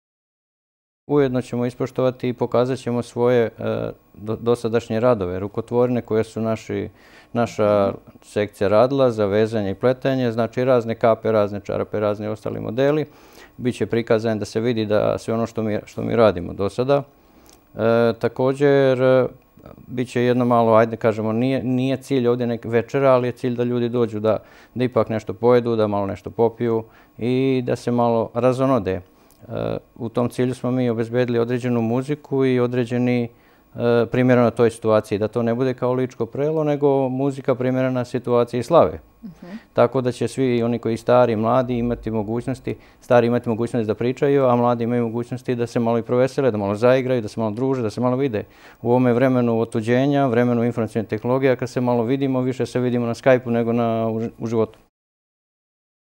Ličani u subotu obeležavaju Mitrovdan
Udruženje Ličana “Lika” u subotu će da obeleži svoju krsnu slavu Mitrovdan. Udruženje je osnovano  05. oktobra 2013. godine, sa ciljem da neguju svoju tradiciju i korene.

Na Mitrovdan, kako dolikuje obeležiće slavu, a prisutnima će biti predstavljena  i kultura iz tih krajeva.